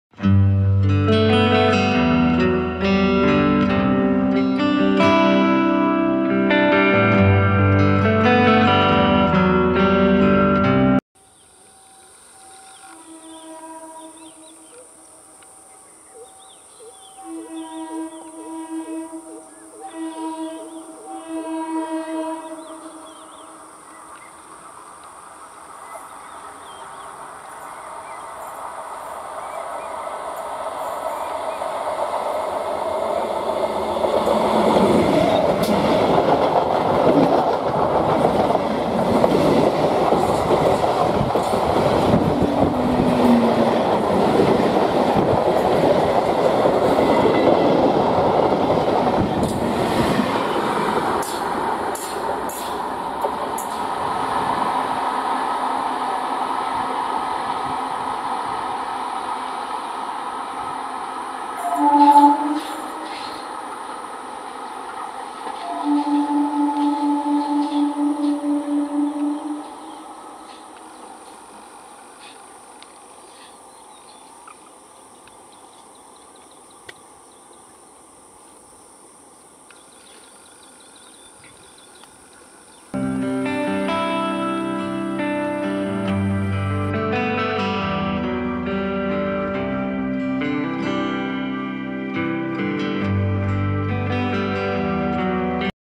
Colourful EMU Train Speeding in a Huge Curve | Medha Livery EMU Local of Eastern Railways
Staff Special Train on Katwa-Bandel Route of Eastern Railways.